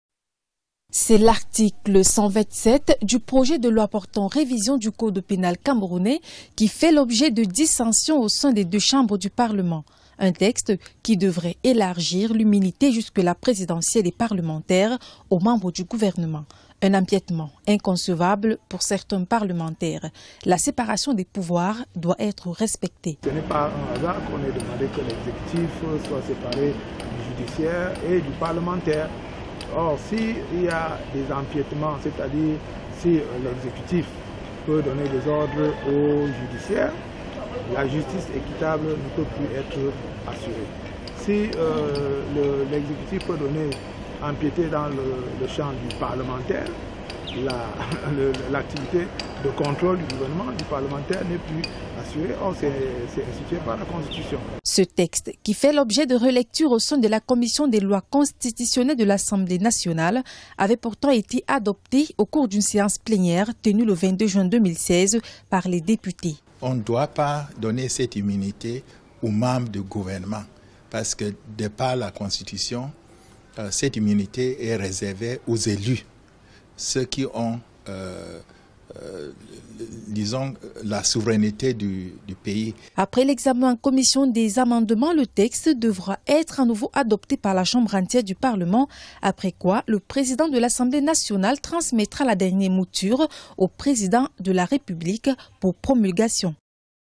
Cameroun, Relecture de la loi sur le code pénal
Politique, Au Cameroun, le projet de loi portant révision du code pénal a connu quelques amendements au cours de la plénière d’adoption du texte par le Sénat à la demande du gouvernement. Au centre de multiples débats, l’article sur les empiètements du judiciaire sur certaines immunités. Notamment celle des membres du gouvernement. Un article qui a entraîné le courroux de certaines législateurs.